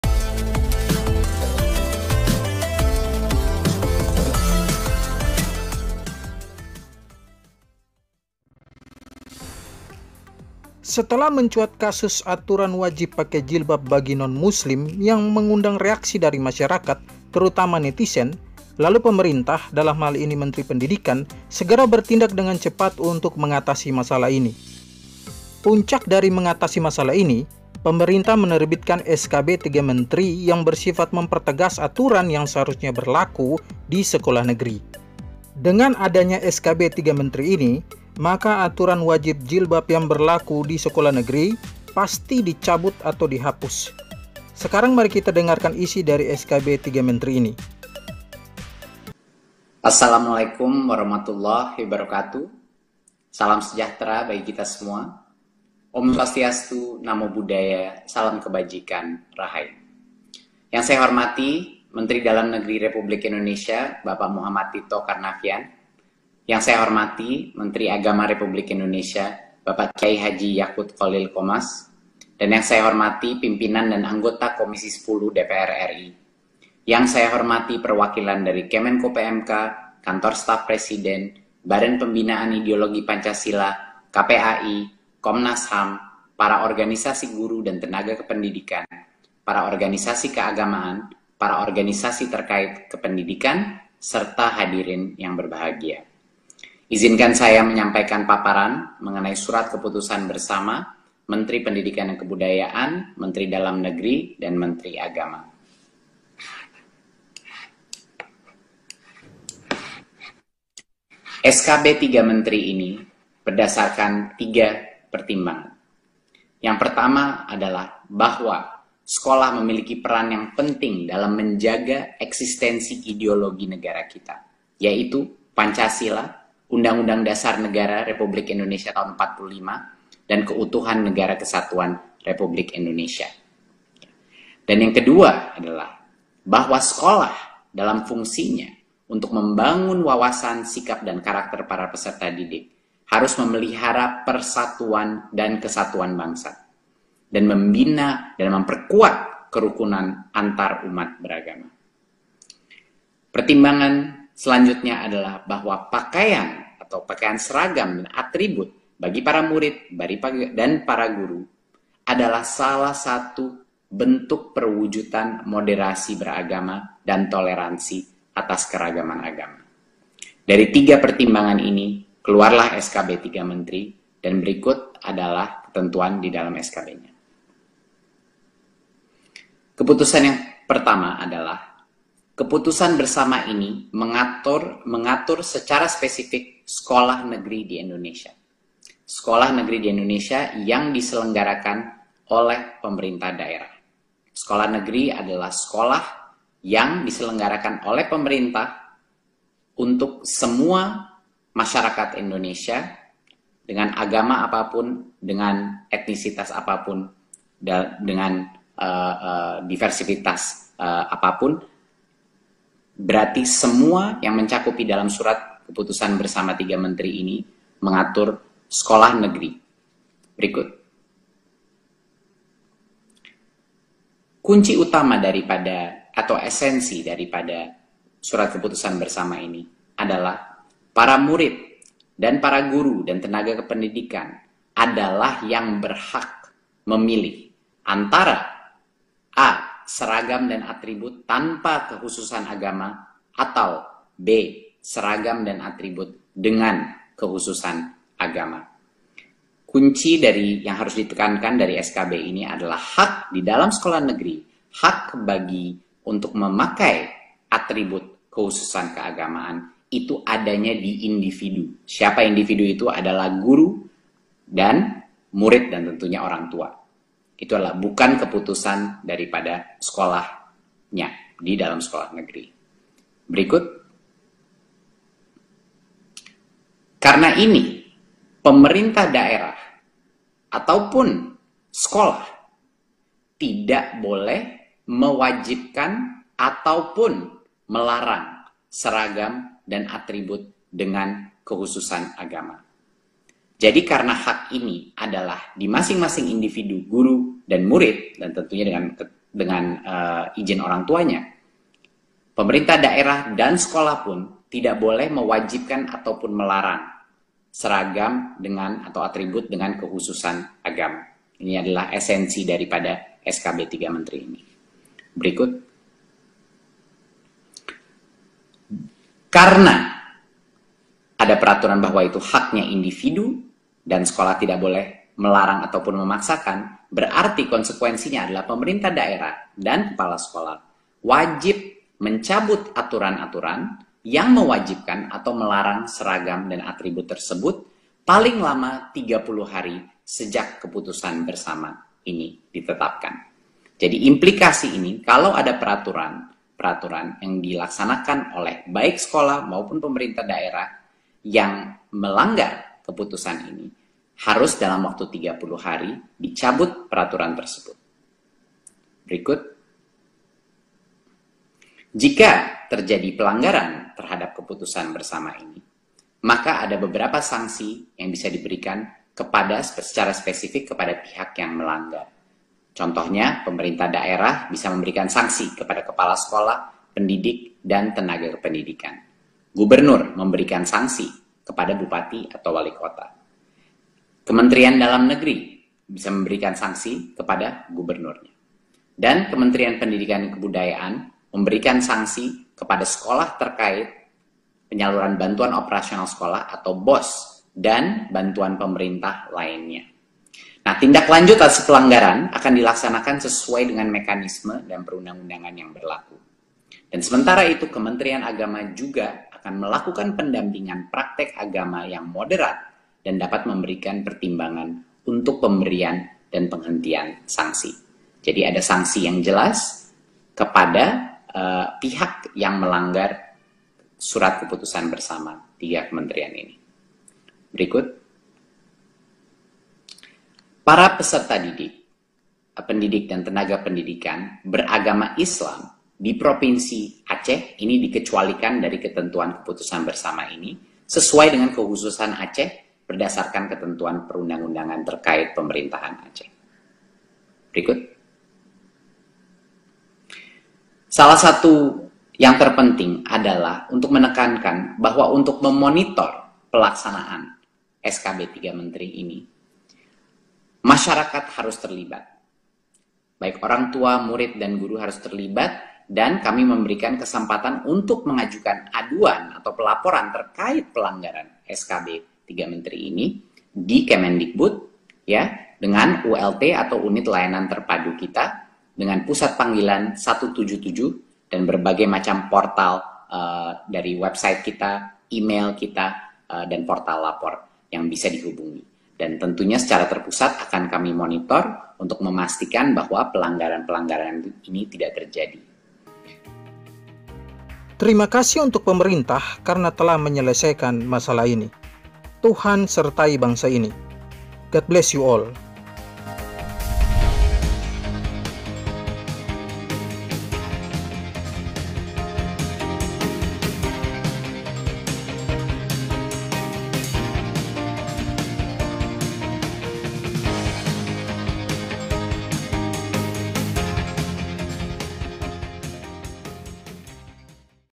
SKB 3 MENTERI TERBIT : Aturan wajib jilbab bagi NON MUSLIM disekolah negeri, WAJIB DICABUT!
Jika saudara-saudara diberkati, bagikanlah video-video dalam channel ini.

TUHAN MEMBERKATI 😇